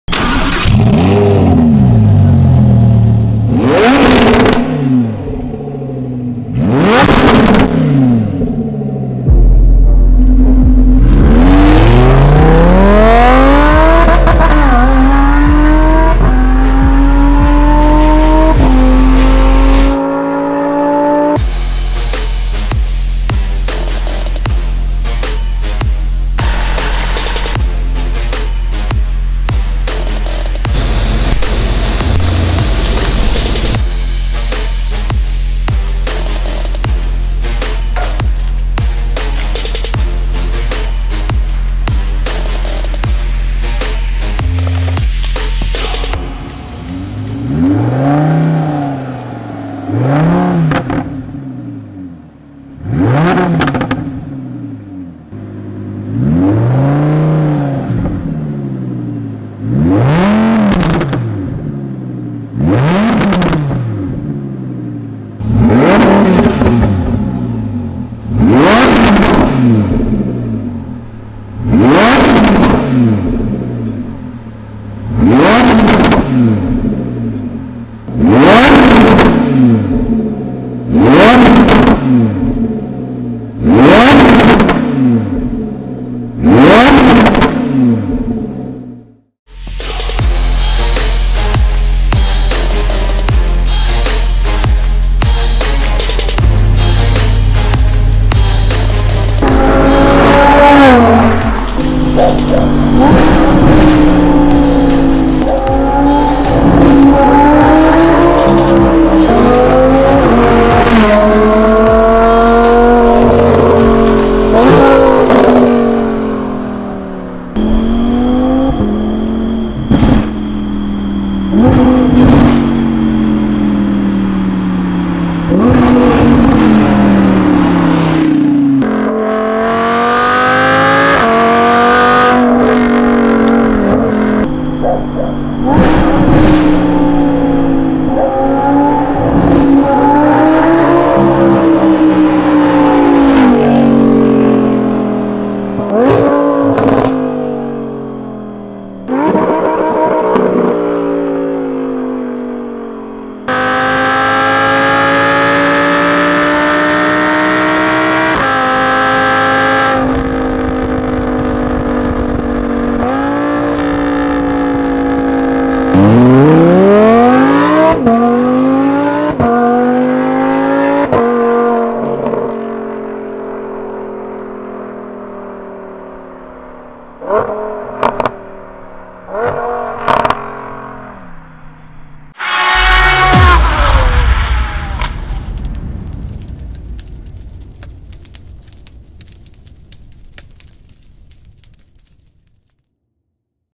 Lamborghini Huracán 610-4 | iPE Exhaust 產品形象廣告影片 & 產品路試原音呈現
本人製作企畫腳本、主導拍攝影片並剪輯成正式的產品宣傳影片，將產品最大的特色 - 聲浪完整展示給消費者。